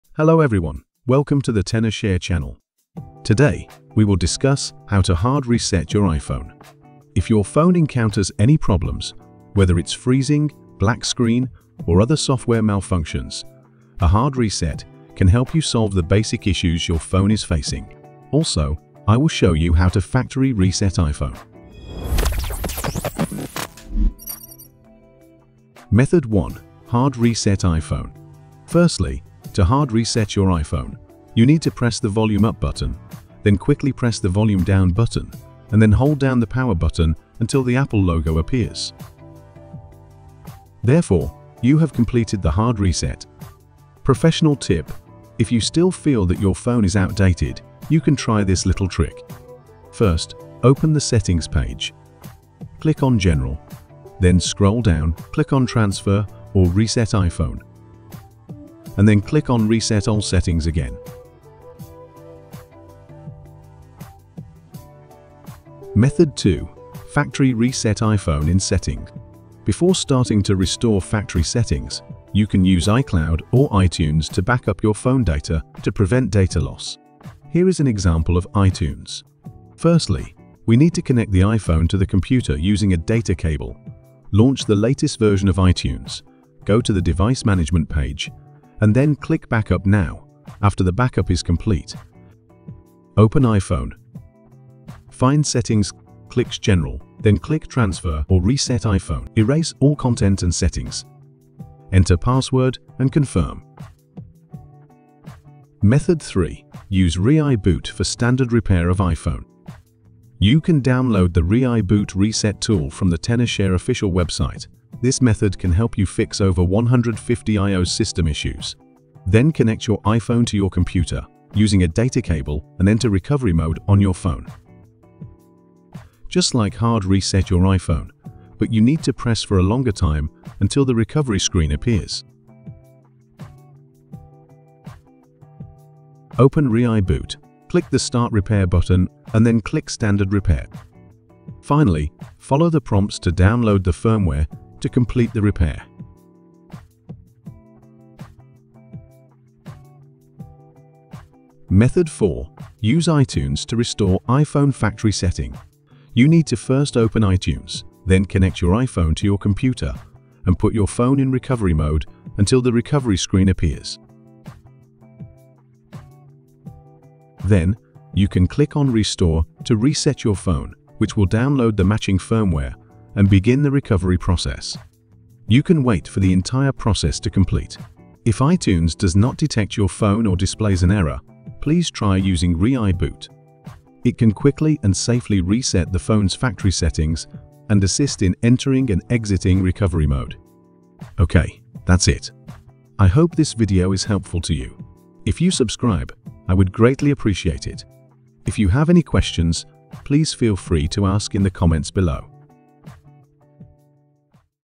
2024 How to Hard Reset iPhone - Apple Support Official Method
Are you upset because your iPhone encounters various black screen crashes and other bugs that cannot be solved? Don’t worry, this video will teach you three methods to quickly solve it and make your phone look like brand new.Let’s dive in!

⏱ *TIMESTAMP*
00:00 Intro
00:25 Method 1: Hard reset iPhone
00:43 Pro tip: Reset all settings
01:06 Method 2: Factory reset iPhone in Settings
01:45 Method 3: Use ReiBoot for standard repair of iPhone
02:31 Method 4: Use iTunes to restore iPhone factory settings